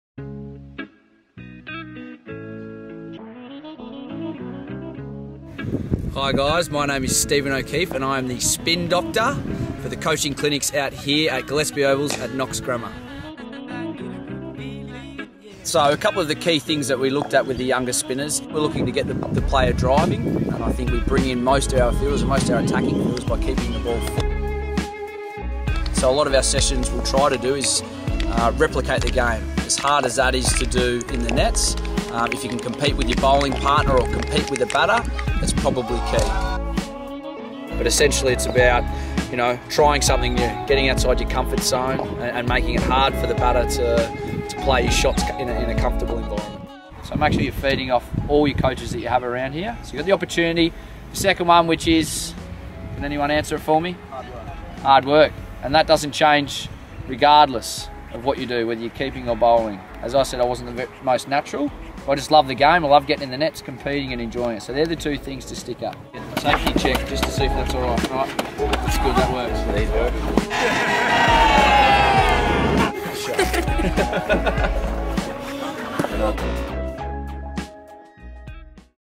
Spin Doctor - Stephen O'Keefe
The MyCoach Cricket Guru Clinic 'Spin Doctor' was run by Australia and NSW left arm spinner, Stephen O'Keefe.

Legend of a man and a super coach.